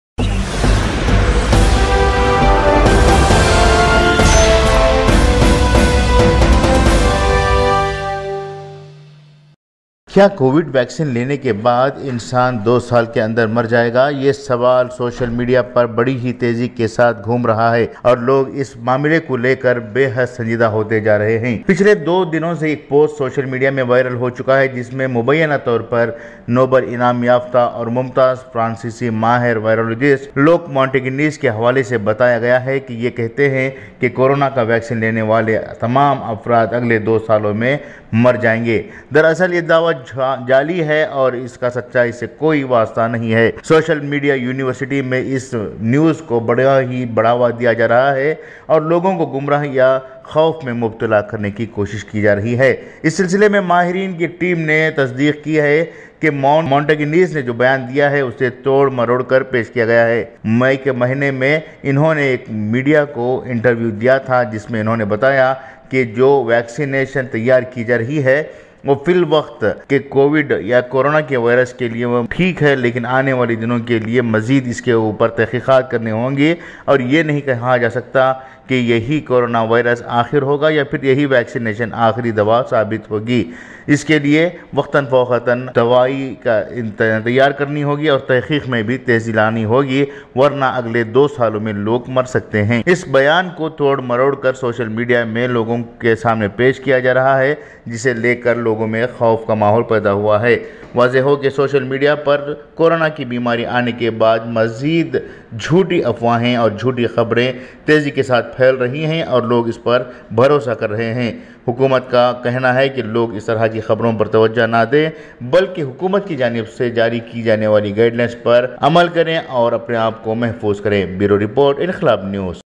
FACT KYA HAI:: VACCINE KAY BAAD 2 SAAL ME MAUT???
According to the official website of the Nobel Prize, French virologist Luc Montagnier was awarded for his contribution to the discovery of the human immunodeficiency virus (HIV) in 2008, however, he has since been in the news for expressing pseudoscientific sentiments. French outlet Science Feedback reported last year that Montagnier inaccurately claimed that the novel coronavirus is man-made and contains genetic material from HIV.